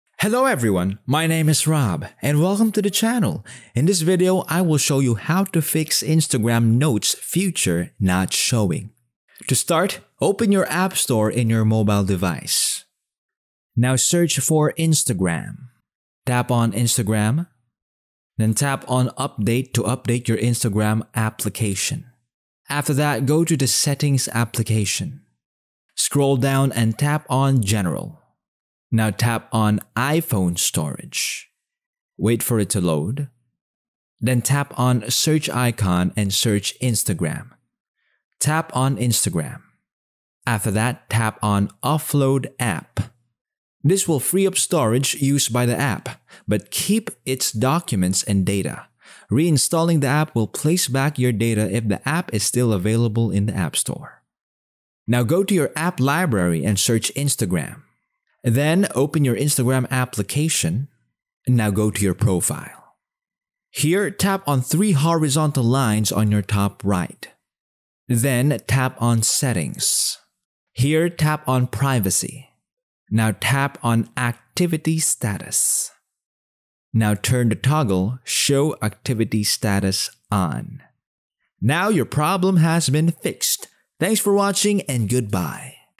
How To Fix Instagram Notes Feature Not Showing ANDROID (EASY 2023)
The Best Of How Too.

In this video I'll show you How To Fix Instagram Notes Feature Not Showing ANDROID. The method is very simple and clearly described in the video. Follow all of the steps & XYZ